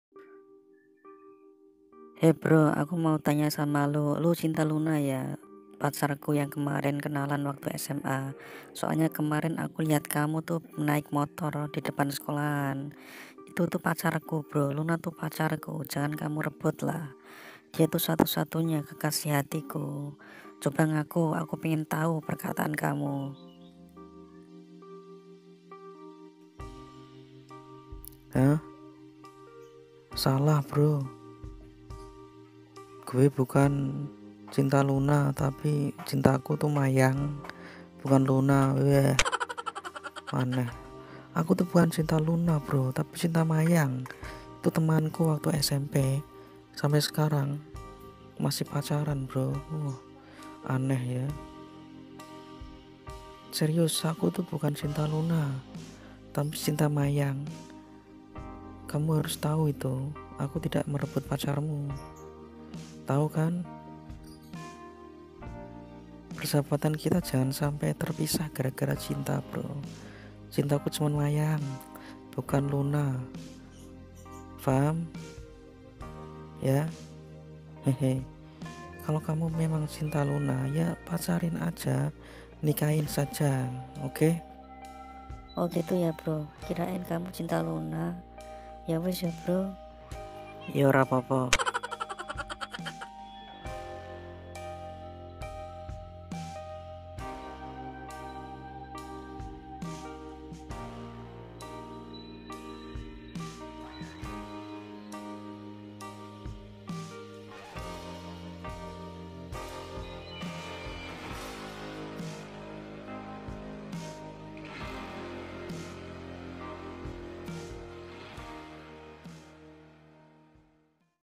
Kartun Lucu : Lucinta Luna? Bukan Tapi Cinta Mayang, Animasi Kartun Lucu Indonesia
Kartun lucu ini terinspirasi dari youtuber kartun teman2, dan ini parodi dari video yang lagi viral dan jadi trending saat ini, tapi parodi kartun lucu ini gak ada niat buat menyindir orang lain kok, cuma hiburan animasi aja dan buat senang senang semoga terhibur ya teman-teman tentang kesalah pahaman lucinta luna, dan ternyata bukan, tapi malah cinta mayang hehee

📢 TURN ON POST NOTIF FOR SHOUTOUT!
KLIK LONCENG/ BEL kamu untuk bisa memanggil ya 🔔

▪ Find Pictures, Updates, And More About Mr Gretz